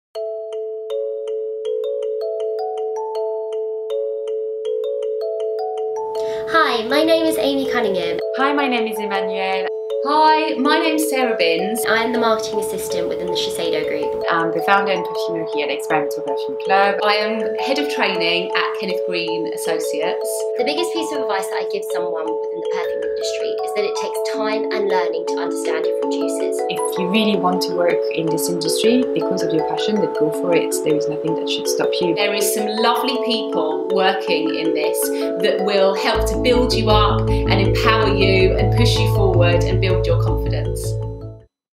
NATIONAL FRAGRANCE WEEK DAY 3!! CAREERS
On day three of National Fragrance Week we bring you some tips and tricks abut how to seek a career in the fragrance industry